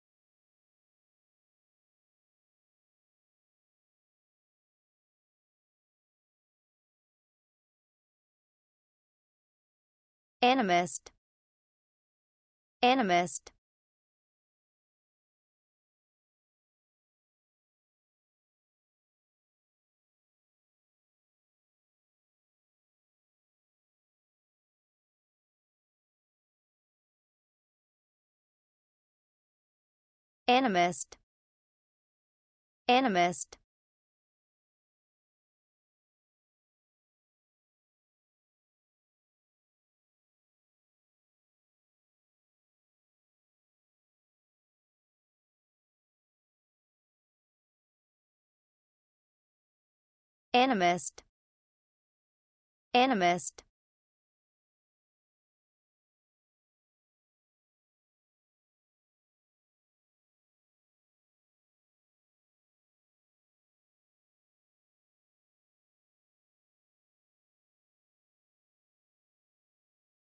How to say Animist in English? - Pronunciation Owl
How to say Animist in English? Learn the pronunciation of Animist!